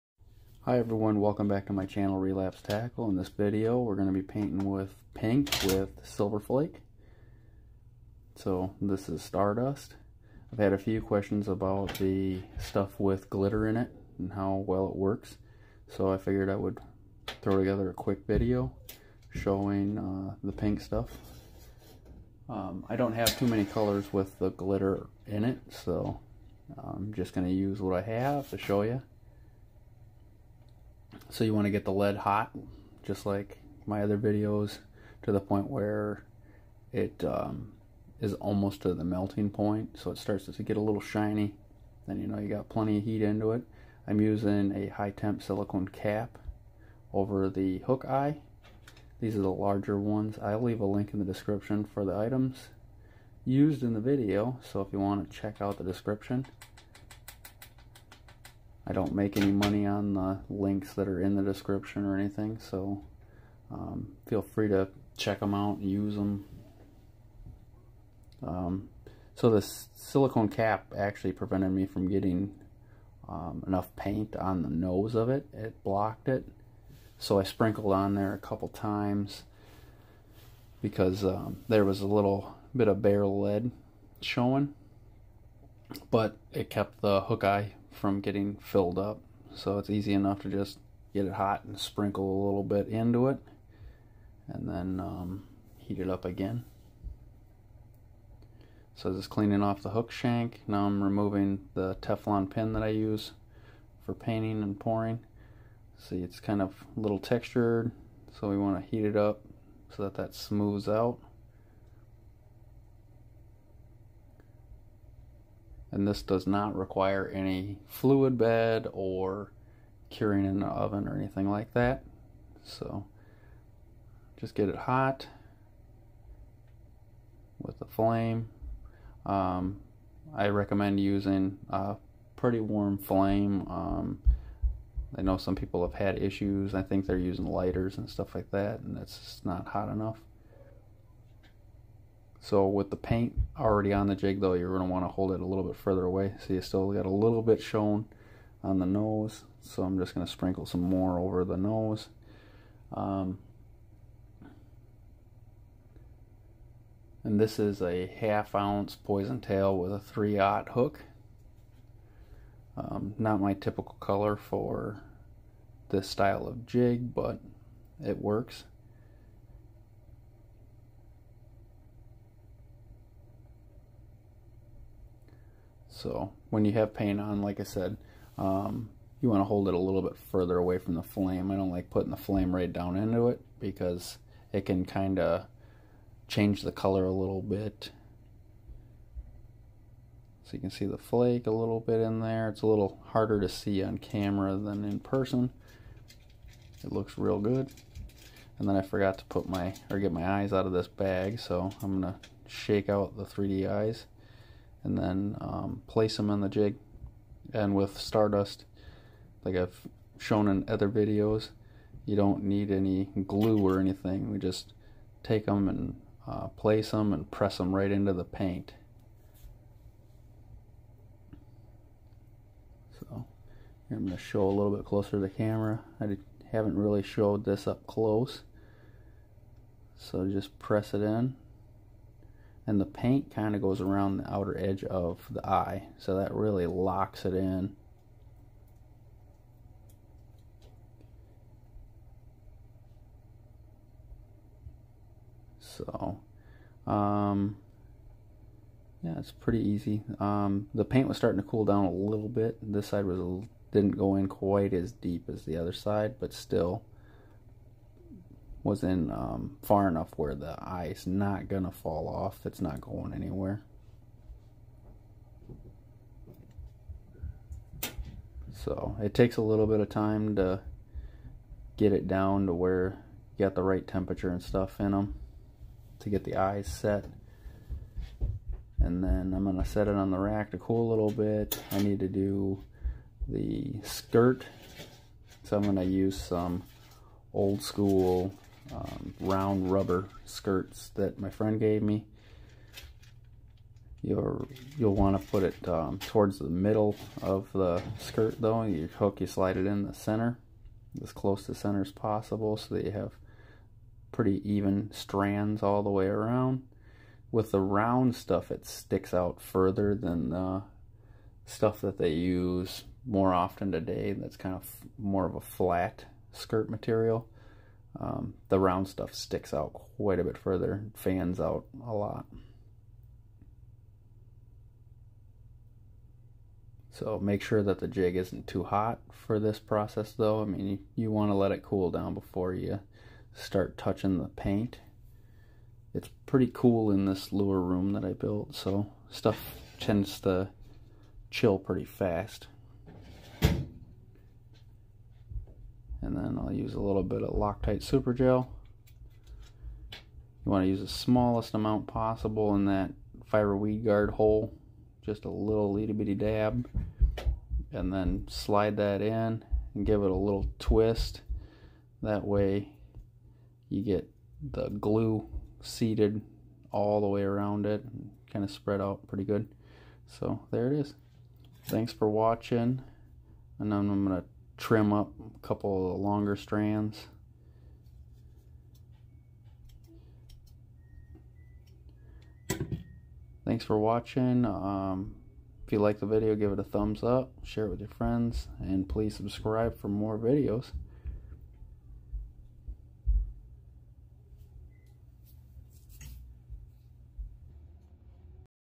Painting a Do-it molded weedless poison tail jig STARDUST Sparkle Pink
If you have any questions feel free to drop a comment, I'd love to hear from you.